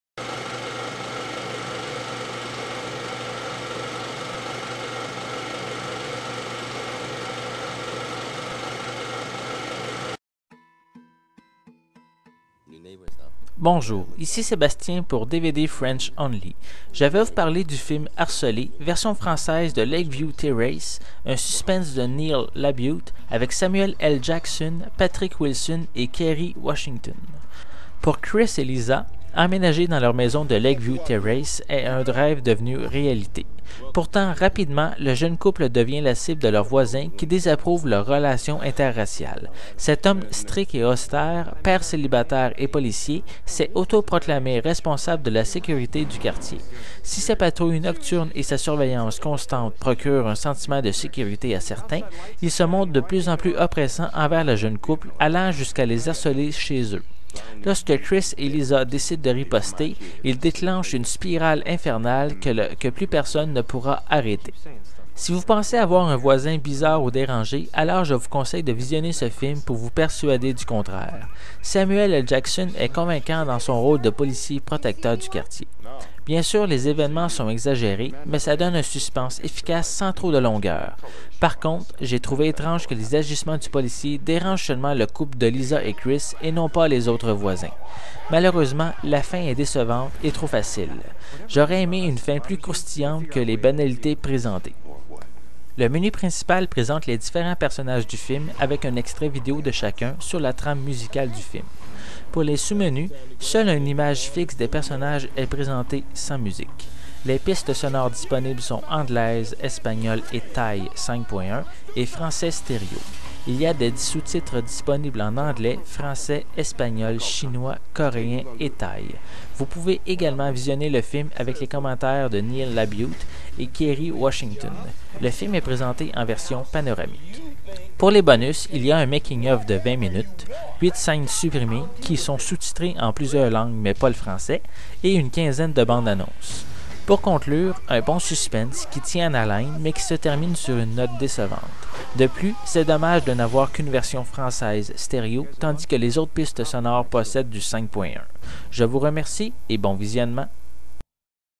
Critique dvd du film Lakeview Terrace
critique dvd du film Harcelés, VF de Lakeview Terrace.  Cette critique a été faite par Sébastien de DVD French Only